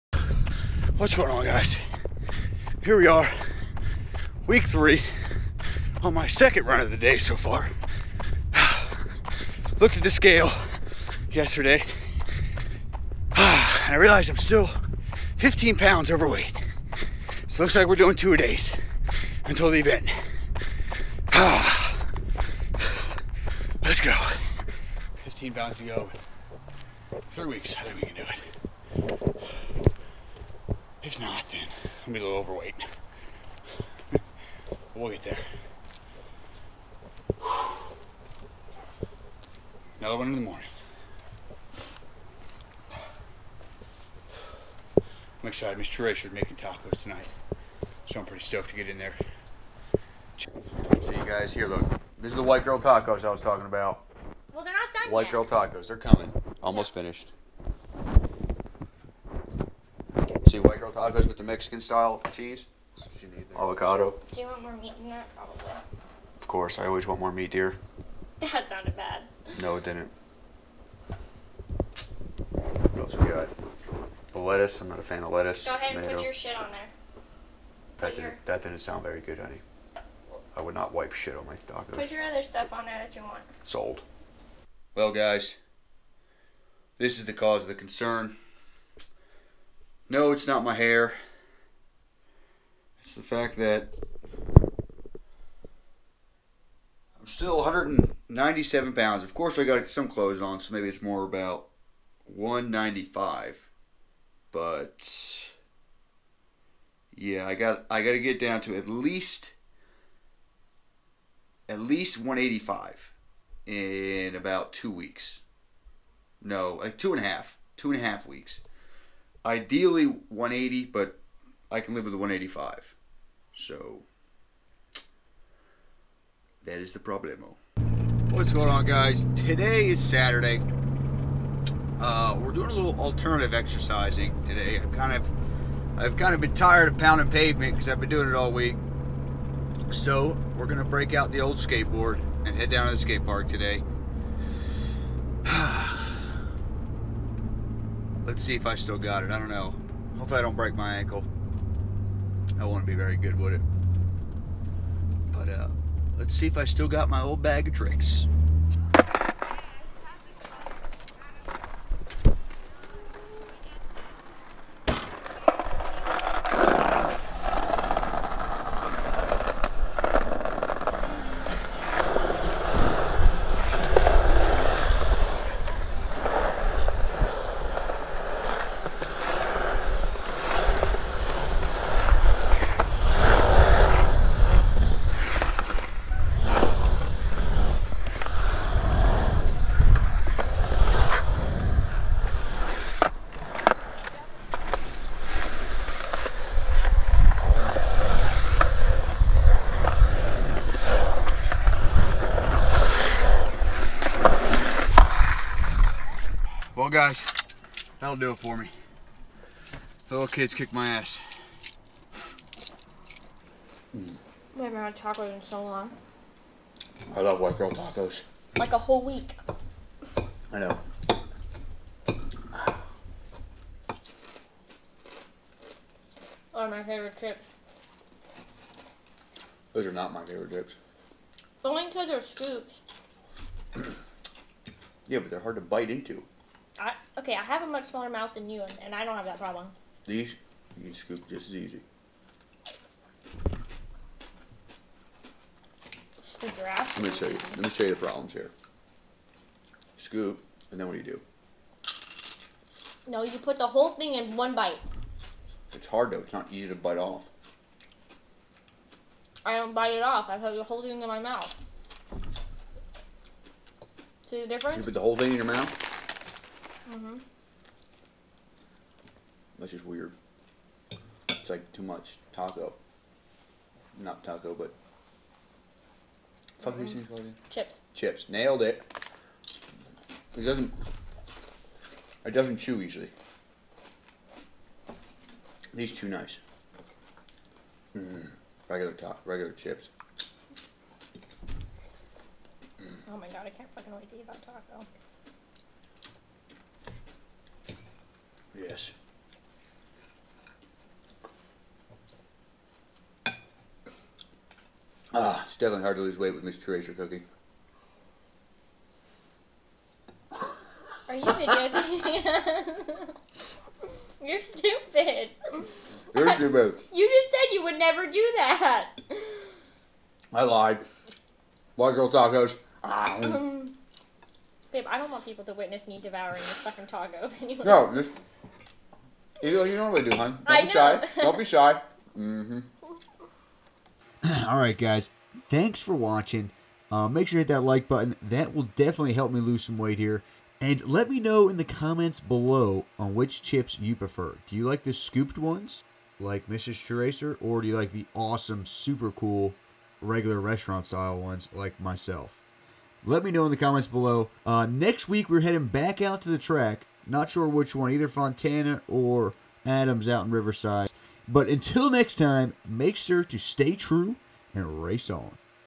The Weight-loss Struggle is Real Vlog #3
With the first race rapidly approaching the struggle to get to optimal weight is getting real. especially with Mrs True Racer cooking.